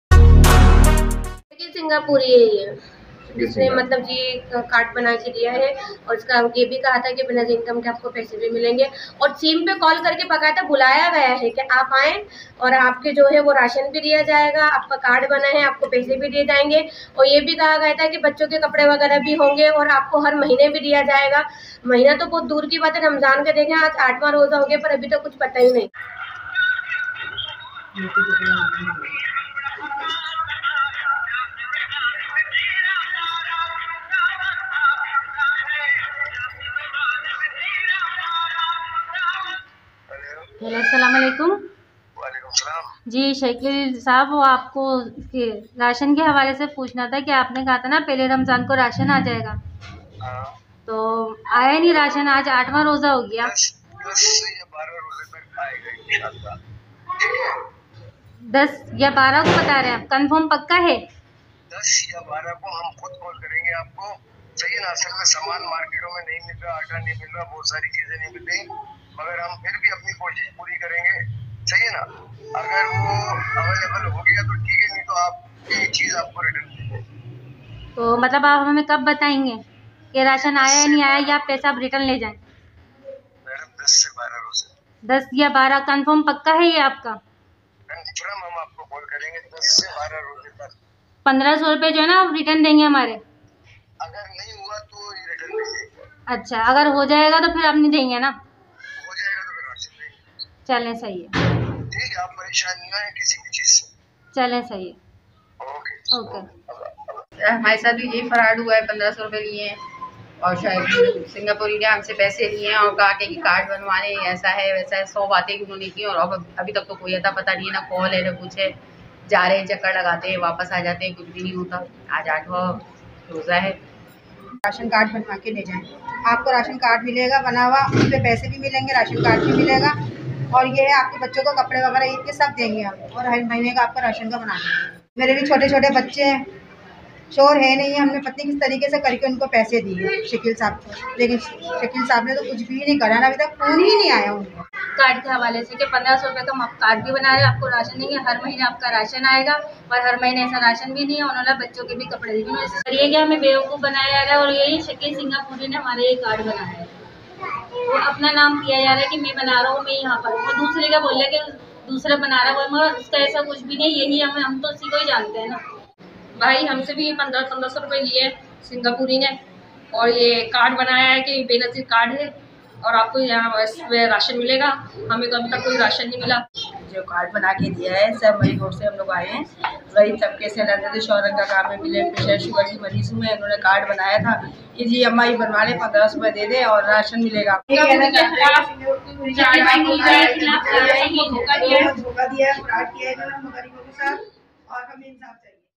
سرجانی ٹاؤن فیز ون کا رہائشی شکیل سنگاپوری رمضان المبارک میں جعلی راشن کارڈ بنا کر عوام کو
سرجانی ٹاؤن فیز ون کا رہائشی شکیل سنگاپوری رمضان المبارک میں جعلی راشن کارڈ بنا کر عوام کو دھوکا دینے لگا عوام کا کہنا ہے کہ شکیل سنگاپوری نے ہم سے راشن کارڈ کے 1500 روپے لیے